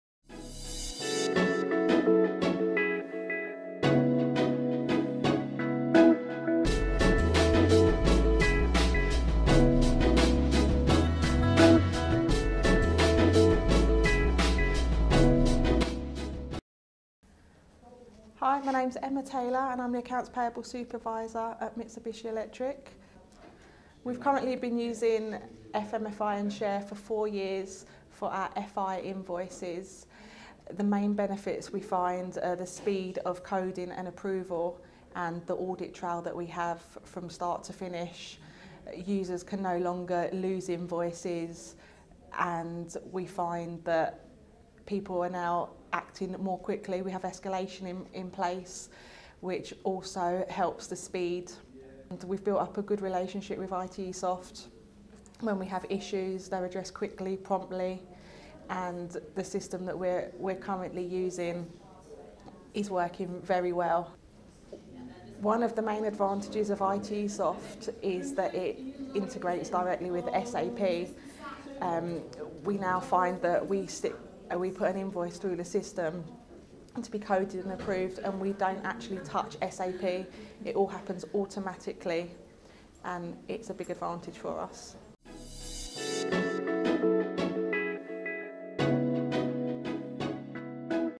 Invoice automation into SAP : Mitsubishi experience
A testimonial from Emma Taylor, accounts payable Supervisor - Mitsubishi